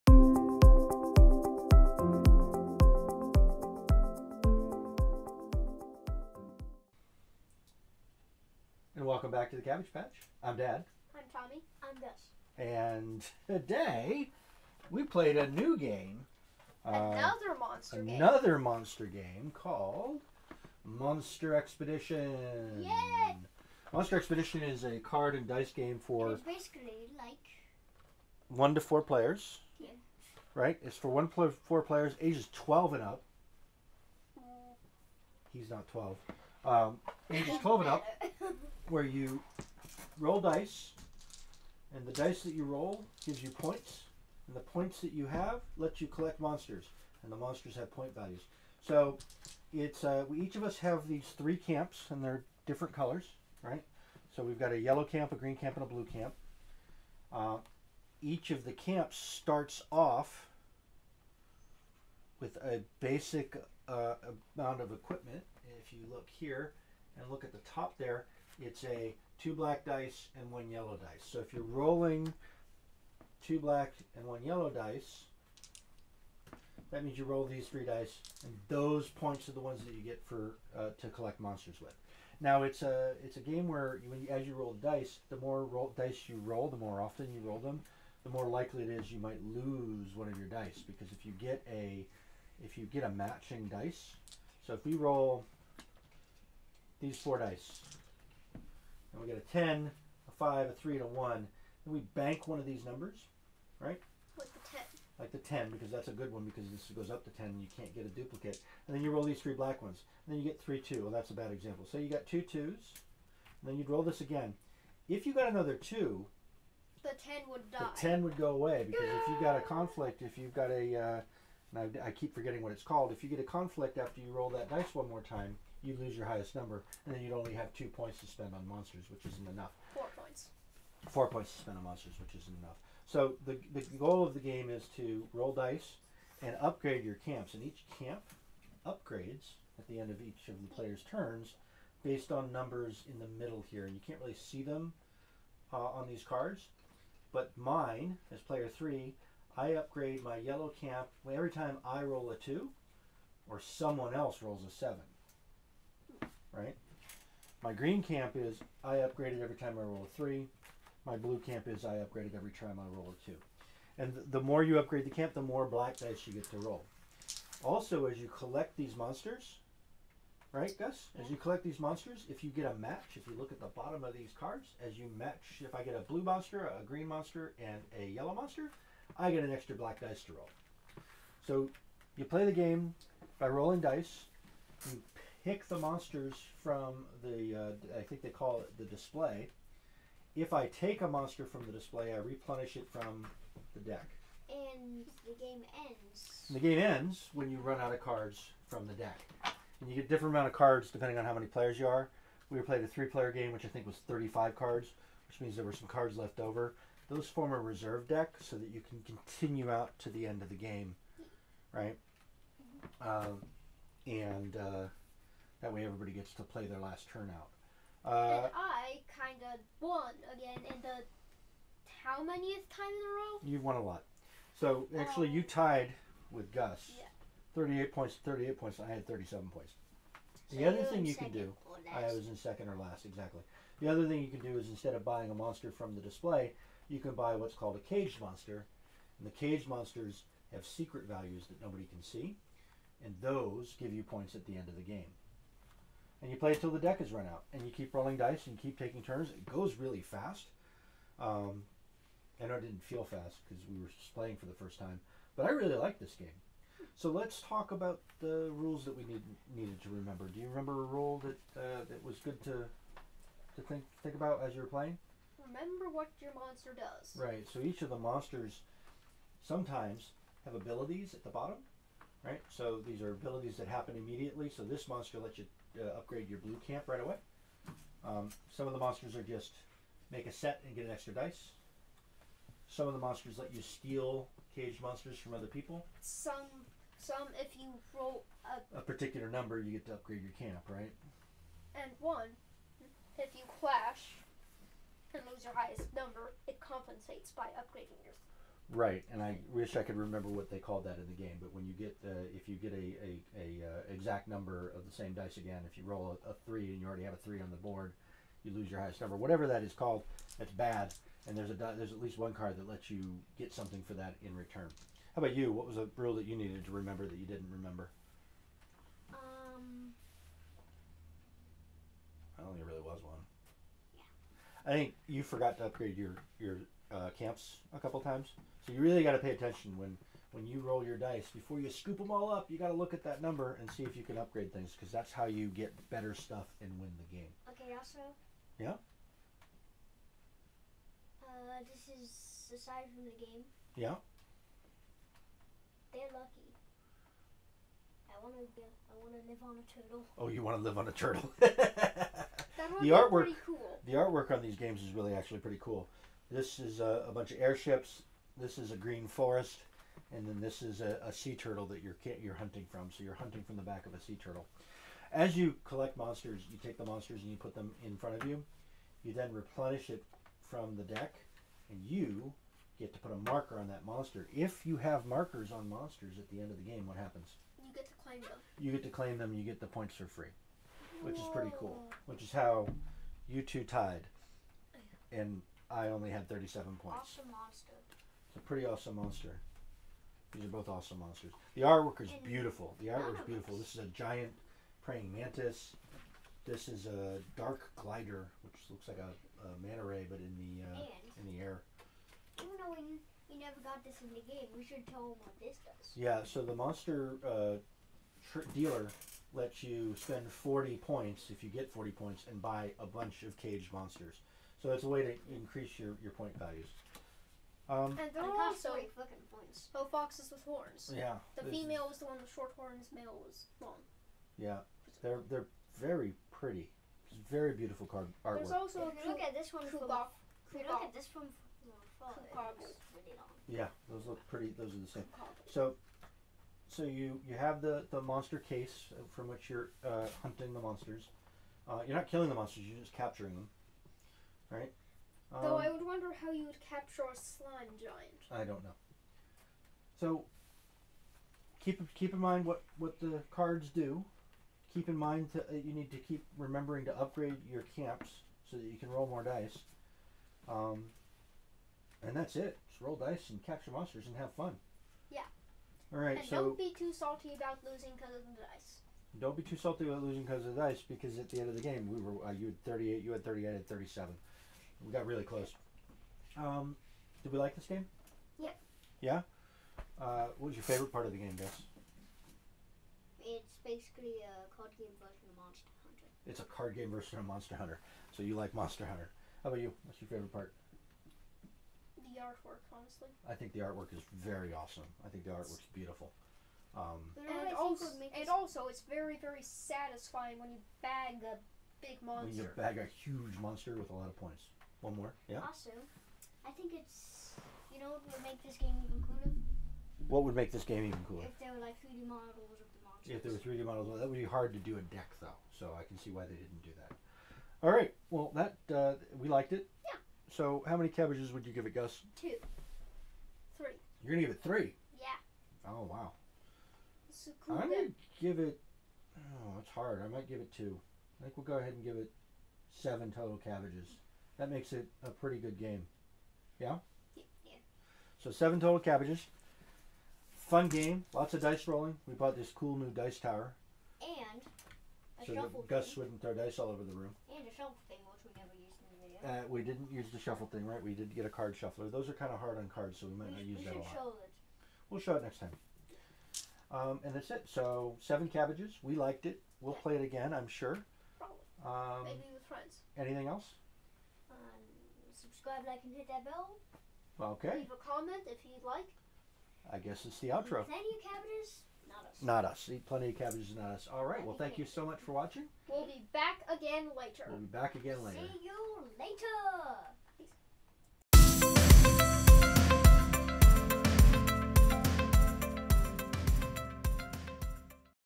Monster Expedition: Cabbage Patch Episode 13
We play the dice rolling Monster Hunting game, Monster Expedition. We liked it a lot, 7 cabbages (I might have held  one back, so really 8).